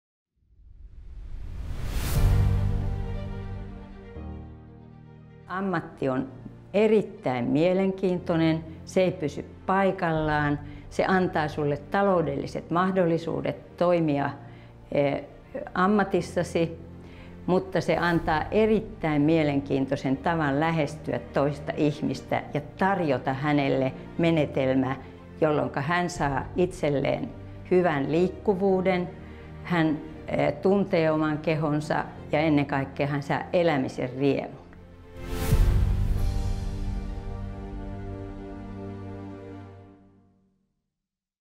Liikkuva ammatti, jossa tarjoat elämisen riemua asiakkaillesi?
Salmella on yli 20 vuotta kokemusta Method Putkisto -ohjaajana. Nyt hän kertoo, mikä tässä työssä ilahduttaa häntä edelleen joka päivä.